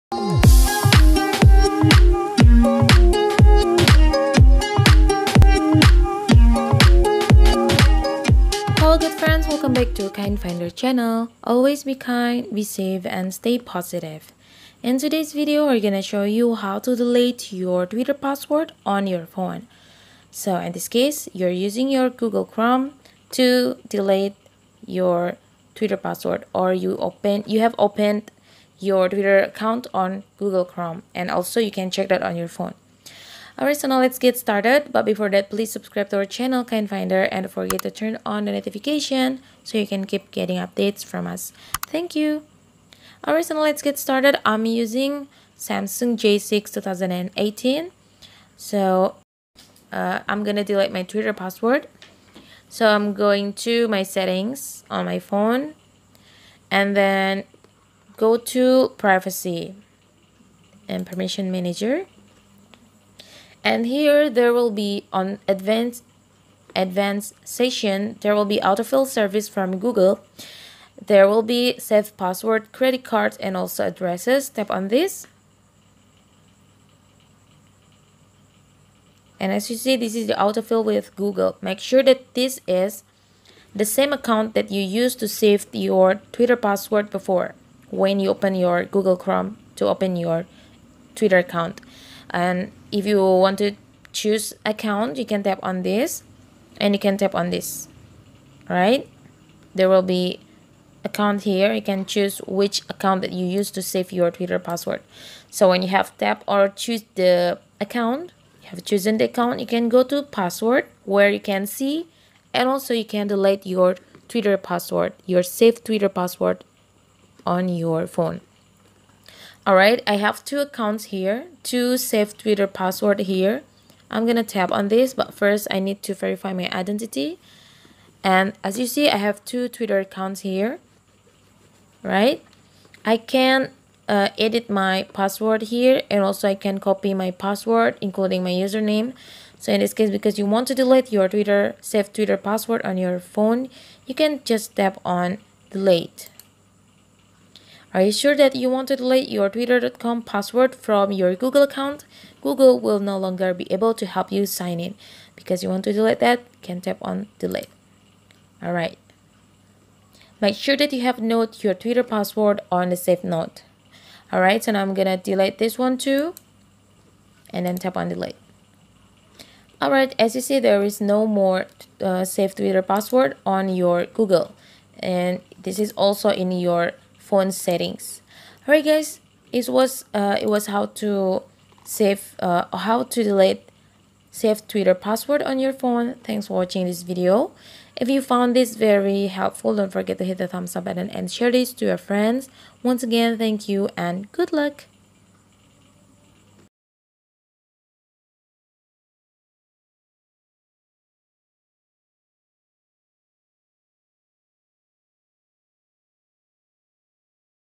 How to delete saved Twitter password on phone
This video shows you How to delete saved Twitter password on phone

If you want to know How to delete saved Twitter password on phone please follow the steps on this video.

Sincerely
Kind Finder